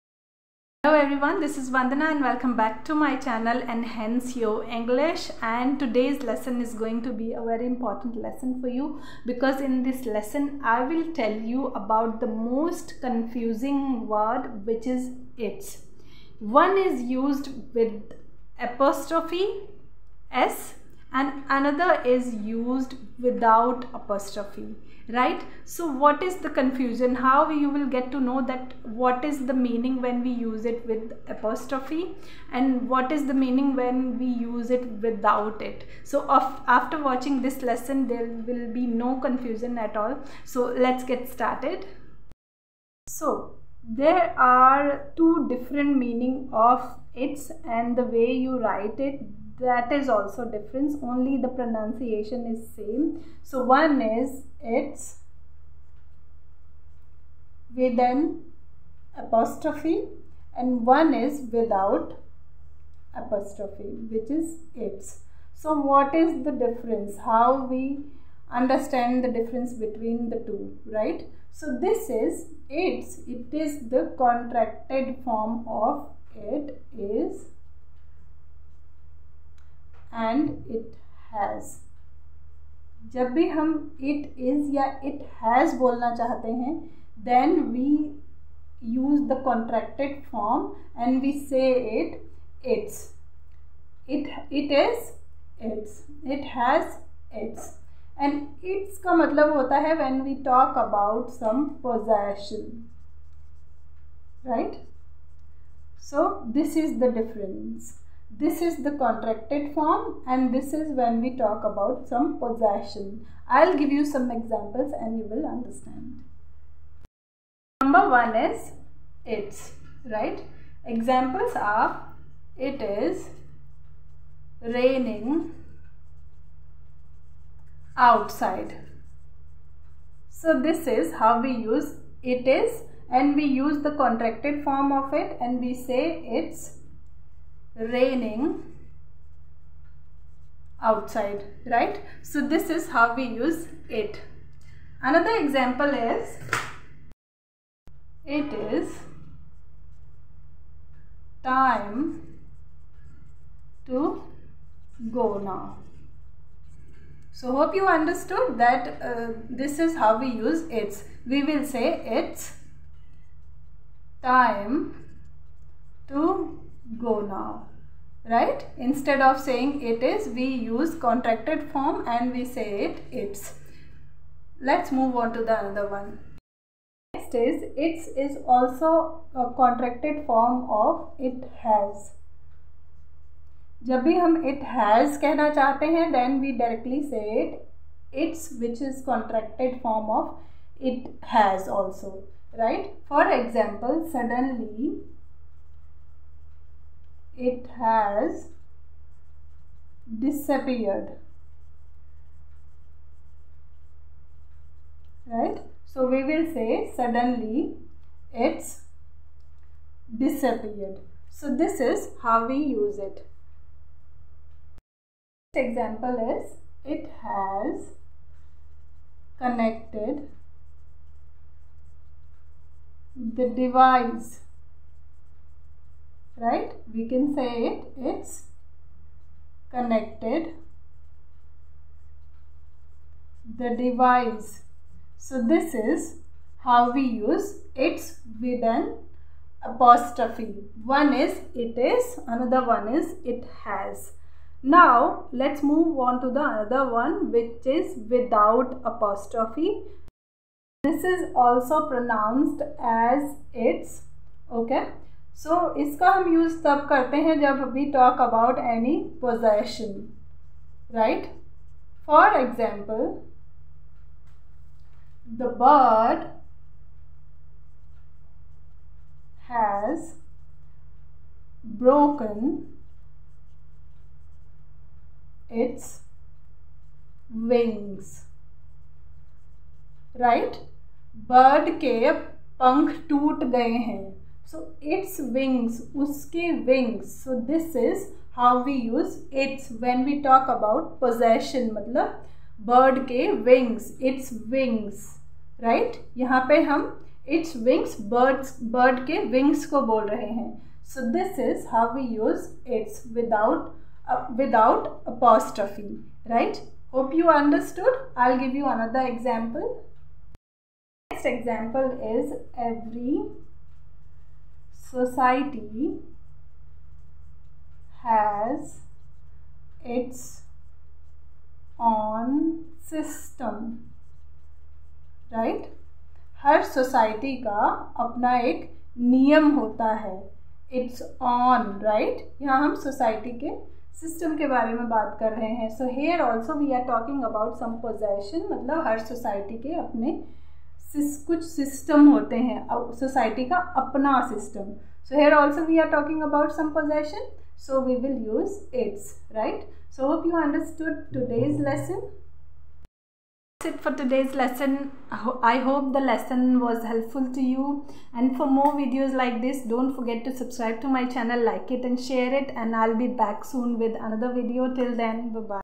Understand what is the difference between It's and It's ?
In this video you will know the diffrence between It's and Its with examples